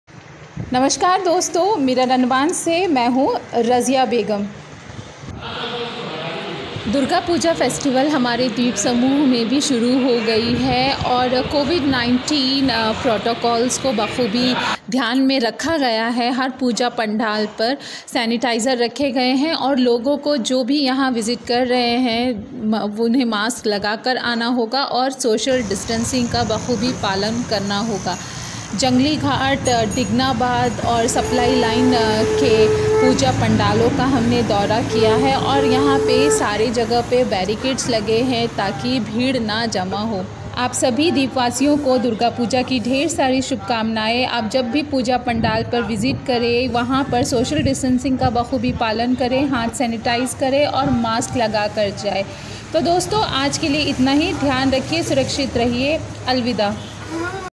Durga puja celebration in A&NI with strict Covid -19 protocols
The annual durga puja festival is started in Andaman and Nicobar Islands , the pooja committees and the devotees have to follow the Covid-19 protocols.